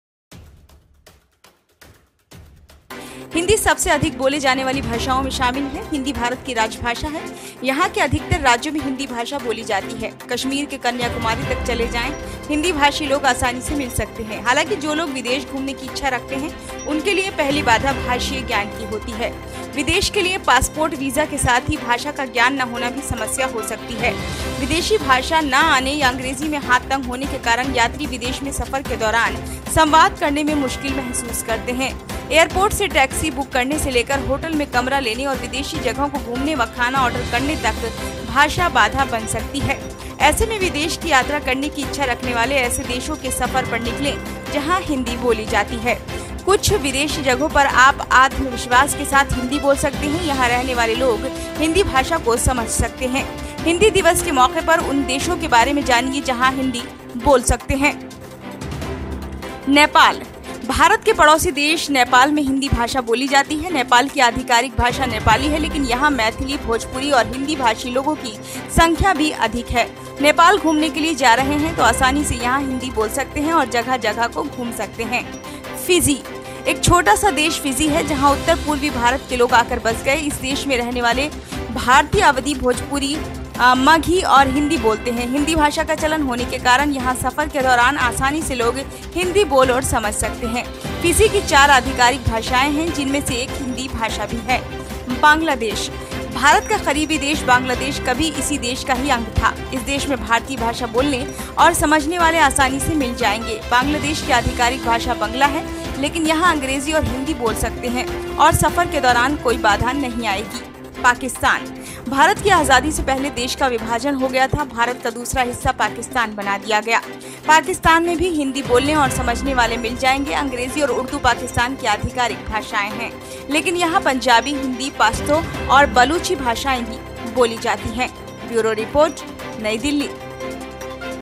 हिंदी दिवस 2023 : इन विदेशी जगहों पर बोलें बेझिझक हिंदी, मिल जाते हैं भाषा समझने वाले
NtvNational  NtvNational  NtvNational

एन टीवी नेशनल...  एन टीवी नेशनल...  एन टीवी नेशनल...